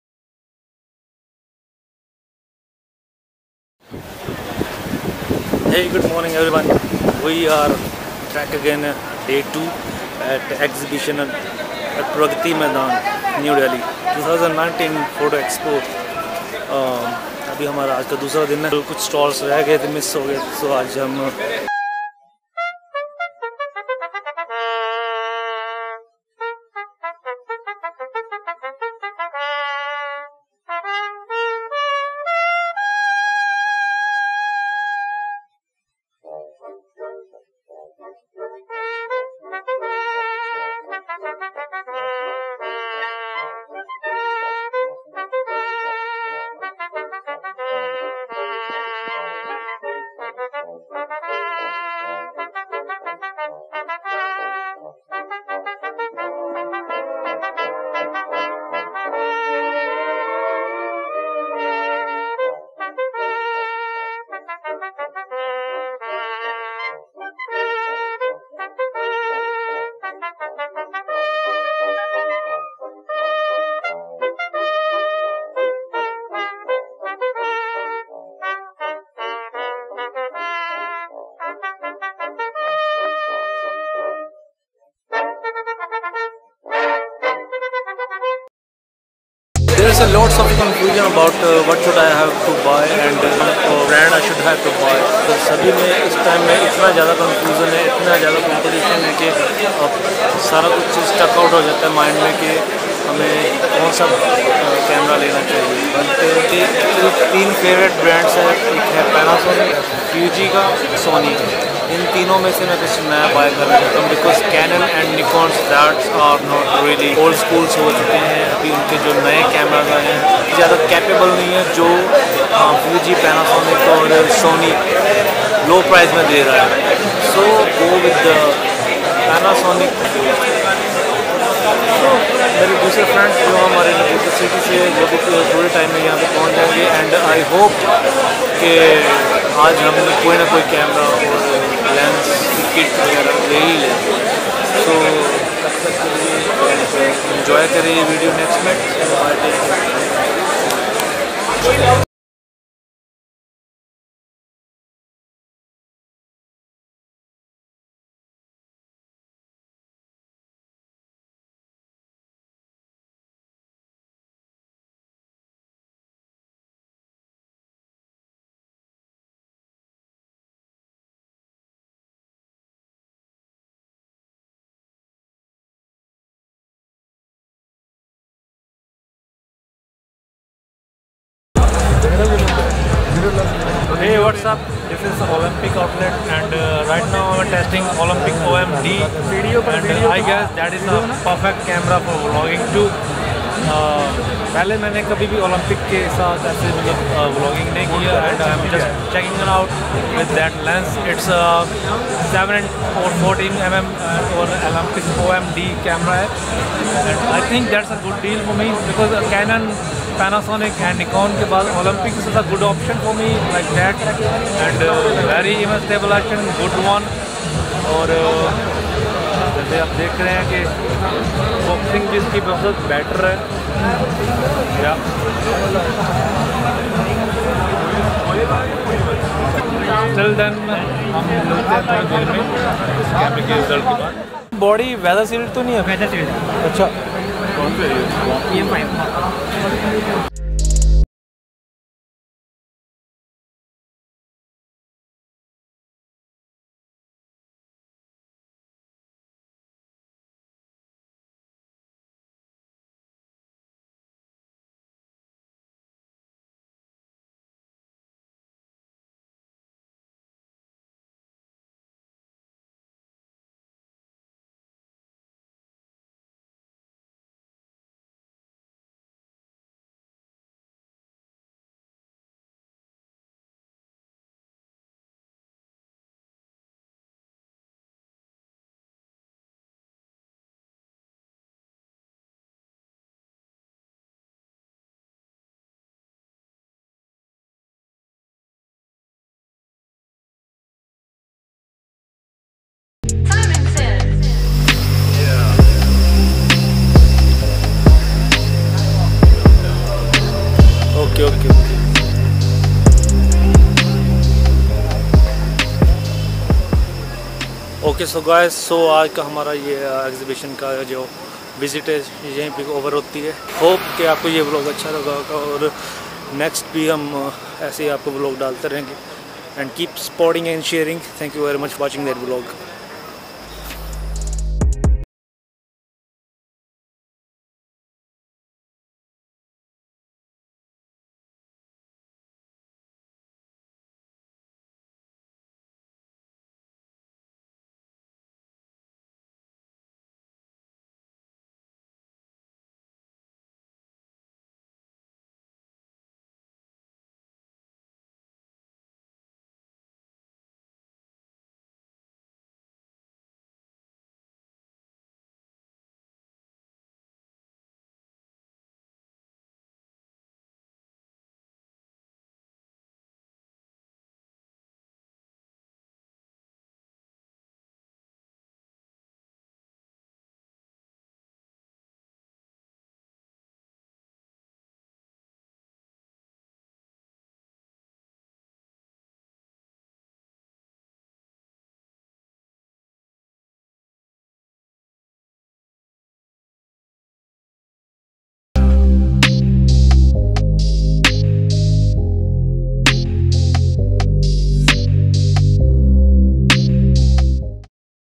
BEST 2019 ASIA PHOTO VIDEO EXPO VLOG PART2 (25,26,27SEP 2019)
Photo Video Asia is proven to be, the leading b2b platform for photography and videography industry, in India, It provides a profound networking environment to all stakeholders.
Photo Video Asia a dedicated high focus platform that provides, the latest in products and technology for high value is niche products & services within India's Photo Market place.
The exhibition's special hall mark is its broad program that covers all major aspects of the photography and videography industry including camera, Video, Printing, Album, Software, Image capturing to image production.
With an amazing array of products and services from all over the world, the show gives visitors an opportunity to source cutting edge products and services as well as learn about and discuss the latest market trends.
Photo Video Asia offers a unique opportunity to engage and network with over 50000 key influences and decision markers working within Photo-Video industry.
An extensive marketing and communication campaign will ensure a high quality trade visitors from across India.

210003
Gears/  gopro Hero7
              YI action cam

Thanks to Sanchit Datta
The Photo Video Asia event is organized for the people like a photographer, videographer, ministries, government agencies etc. which focuses exclusively on the photo and video industries. The exhibition acts as a comprehensive resource for the industry, its members, prospective business visitors and customers by providing in-depth information about the industry and the latest trends influencing its progress.